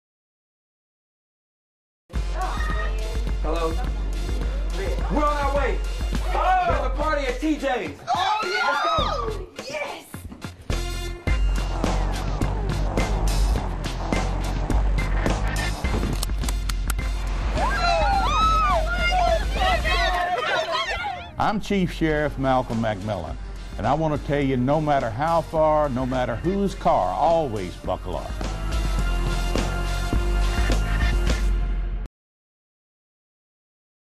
Jackson Teen Seatbelt Coalition video featuring JSU Maddrama Students No. 2
JSU Maddrama students featured in recent Jackson Teen Seatbelt Coalition commercial